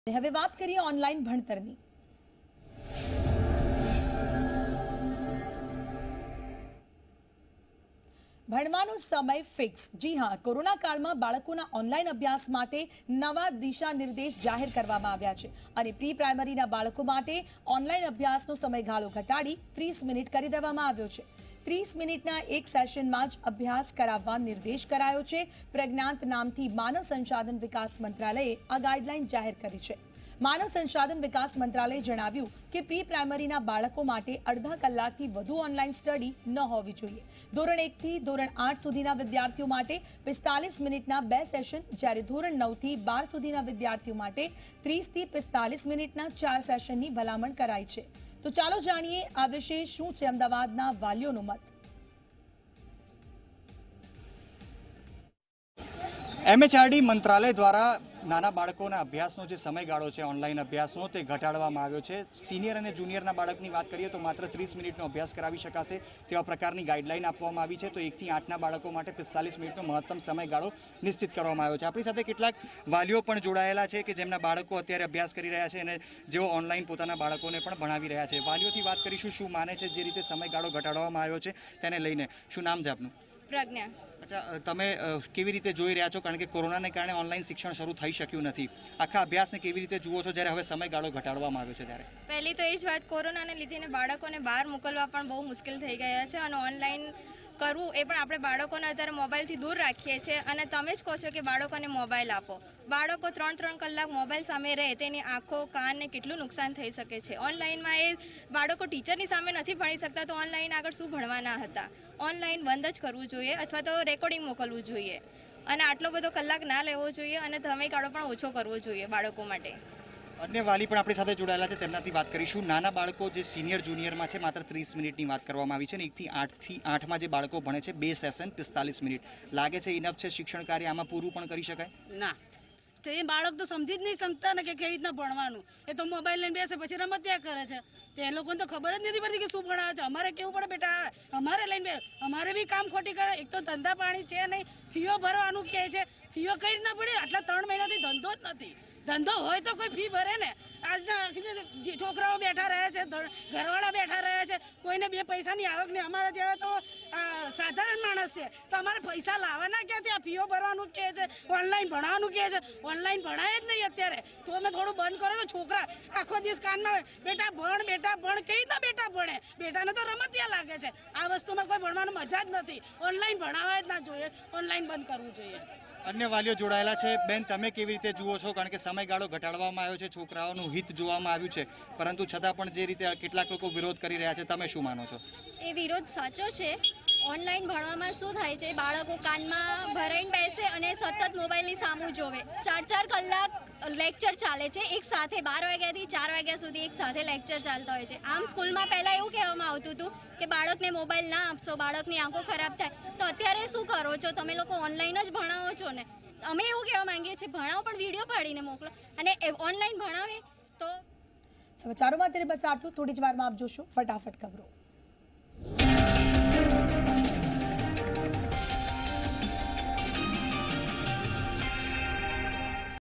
ઓનલાઈન અભ્યાસ | અમદાવાદ વાલીઓના મત | Online Education new Guidelines | ZEE 24 કલાક | Zee 24 Kalak
કોરોનાકાળમાં બાળકોના ઓનલાઈન અભ્યાસ માટે નવા દિશાનિર્દેશ જાહેર કરાયા છે. પ્રિ-પ્રાઈમરીના બાળકો માટે ઓનલાઈન અભ્યાસનો સમયગાળો ઘટાડીને 30 મિનિટ કરી દેવાયો છે. ધો.1 થી 8 સુધીના વિદ્યાર્થીઓ માટે 45 મિનિટના બે સેશન જ્યારે ધો.9થી 12 સુધીના વિદ્યાર્થીઓ માટે 30થી 45 મિનિટના 4 સેશનની ભલામણ કરાઈ છે.ચાલો જાણીએ આ વિશે શું છે અમદાવાદના વાલીઓનો મત...